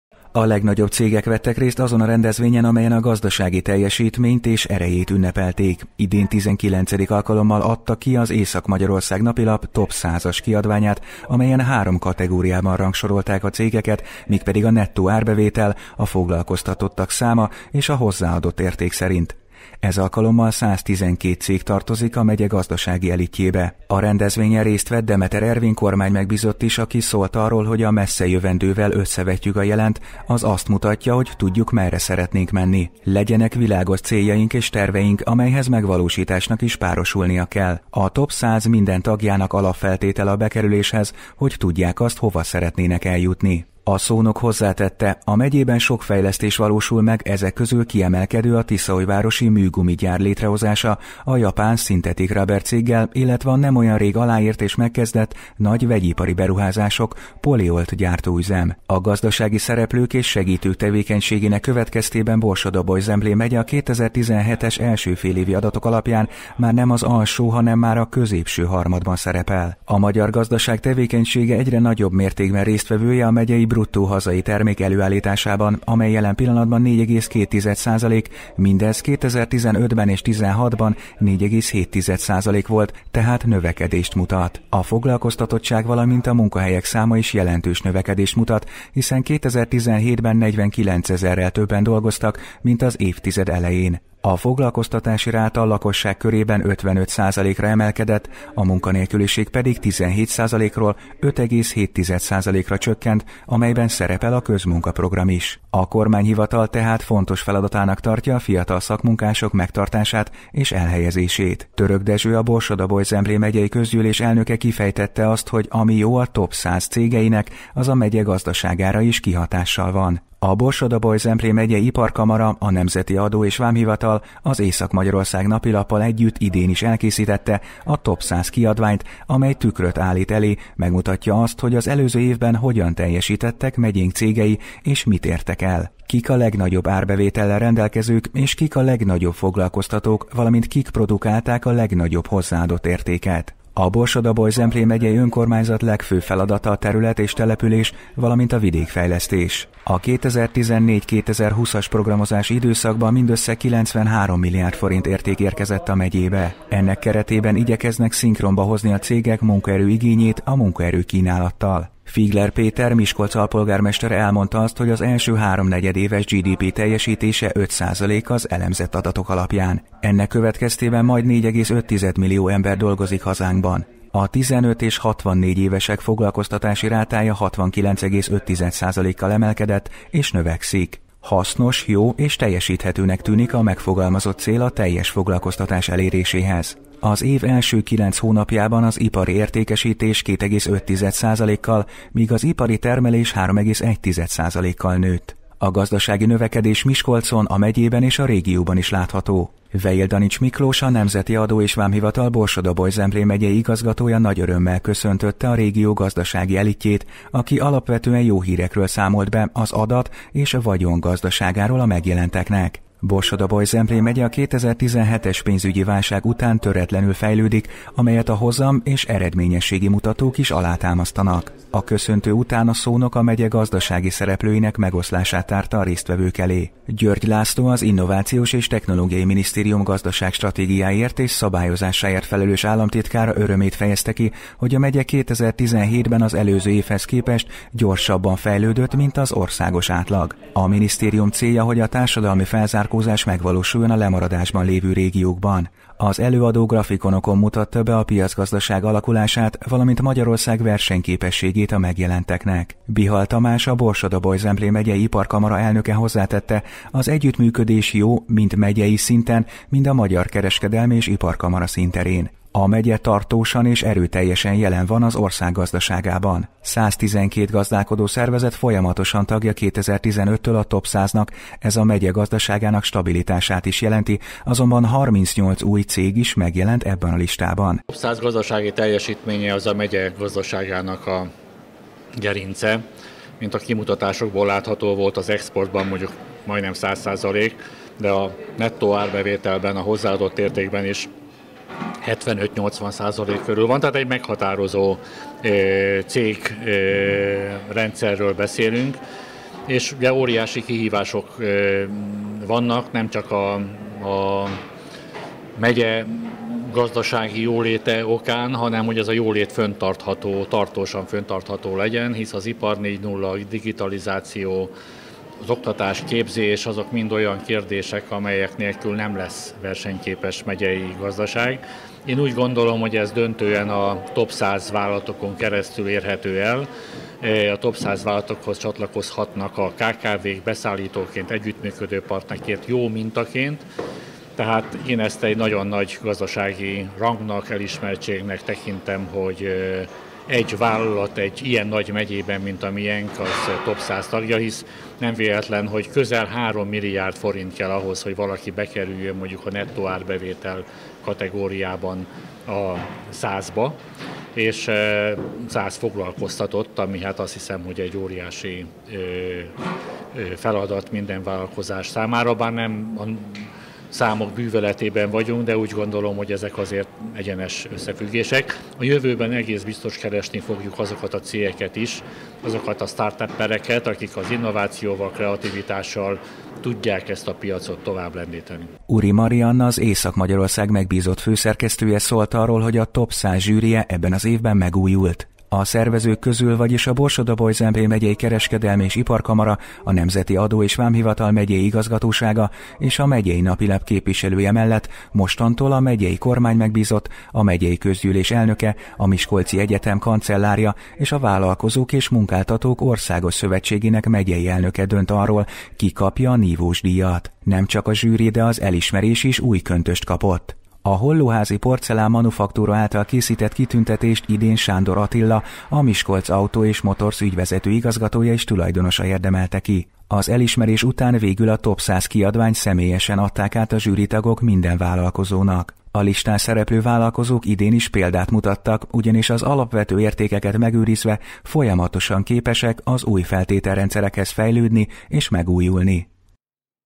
A MEGYE LEGJOBB VÁLLALKOZÁSAIT DÍJAZTÁK
Mint minden évben idén is megtartották a TOP 100 rendezvényt ezúttal november 19-én, a lillafüredi Palotaszállóban. A Borsod-Abaúj-Zemplén Megyei Kereskedelmi és Iparkamara, a Nemzeti Adó-és Vámhivatal, valamint az Észak-Magyarország napilap közös munkájának köszönhetően ünnepélyes keretek között mutatták be a megye száz legjelentősebb vállalkozásának összesített adatait tartalmazó kiadványt.